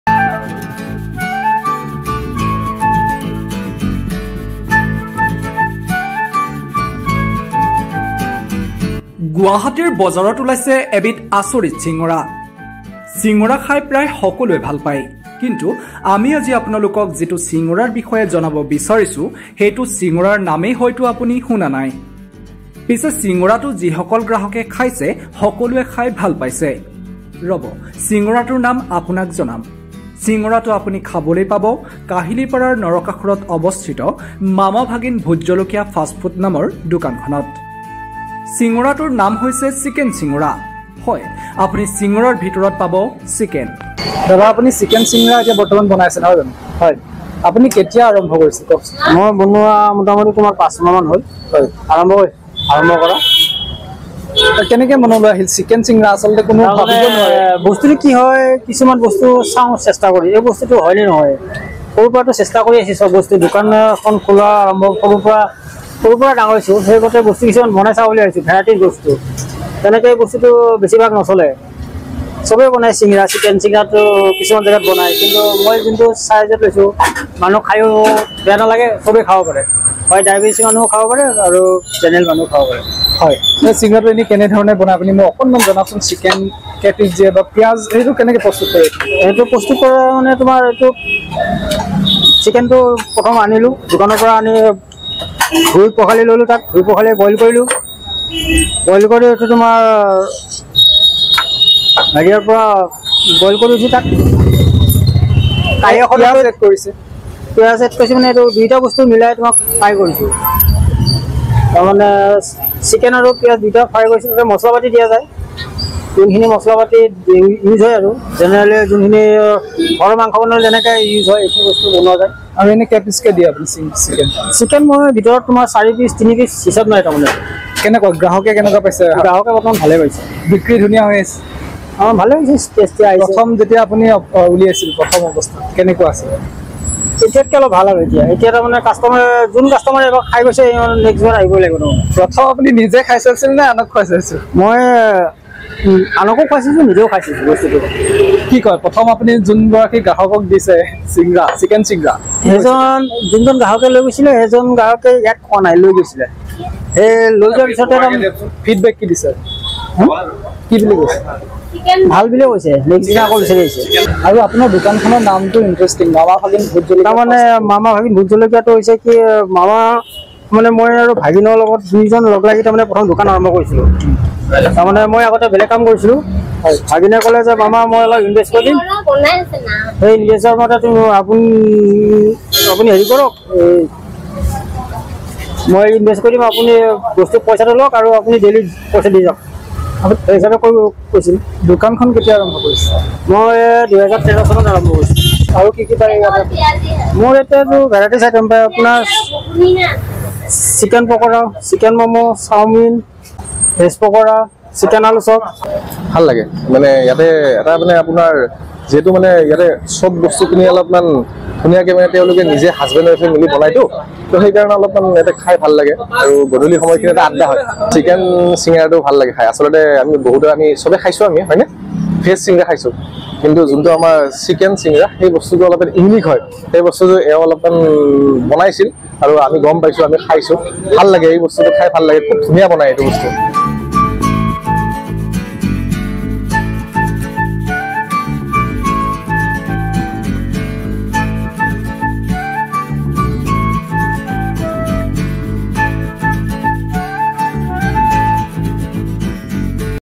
Guwahati News: গুৱাহাটীত ওলাইছে এবিধ আচৰিত চিঙৰা, আপুনি নামেই শুনা নাই এইবিধ চিঙৰা
Guwahati News: গুৱাহাটীত ওলাইছে এবিধ আচৰিত চিঙৰা, আপুনি নামেই শুনা নাই এইবিধ চিঙৰাৰ